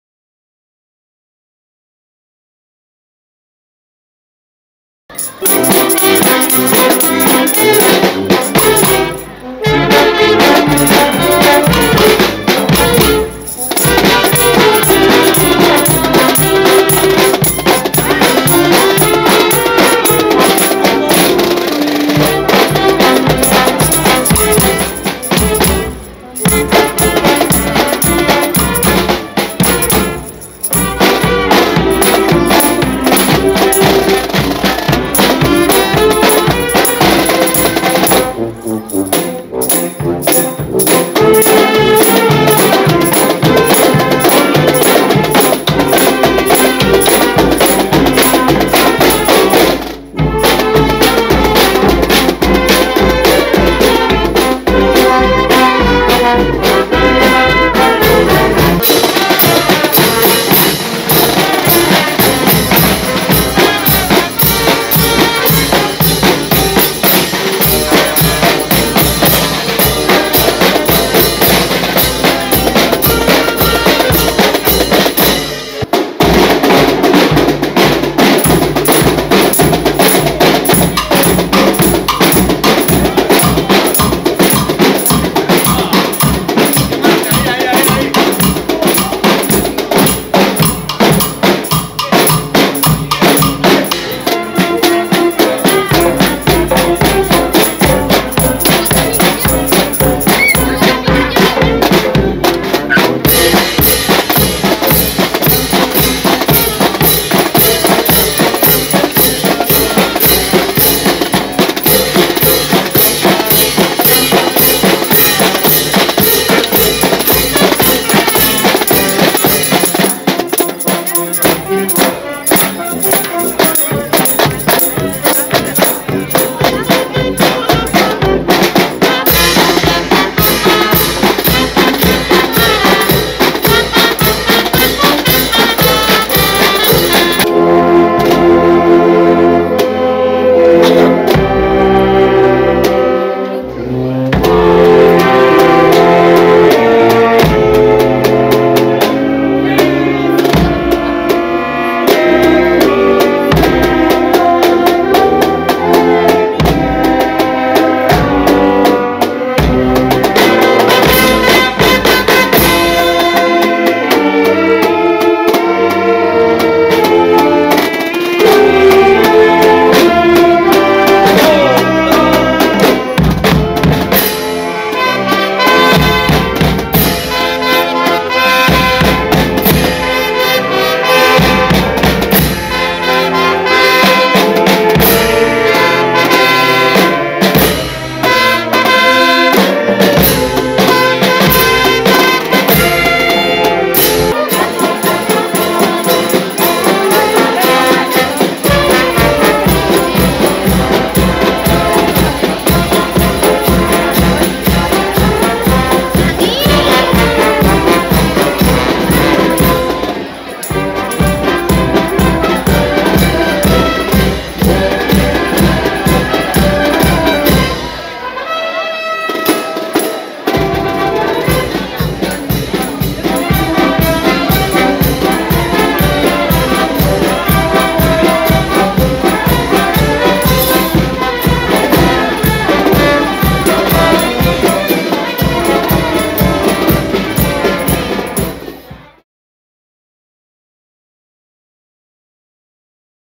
Pasacalles de Navidad de la Banda Municipal de Música
Pasacalles con el que la Banda de Música Municipal animó el ambiente navideño de Güéjar Sierra con algunos de los villancicos tradicionales más conocidos que pusieron la nota musical a estas fiestas.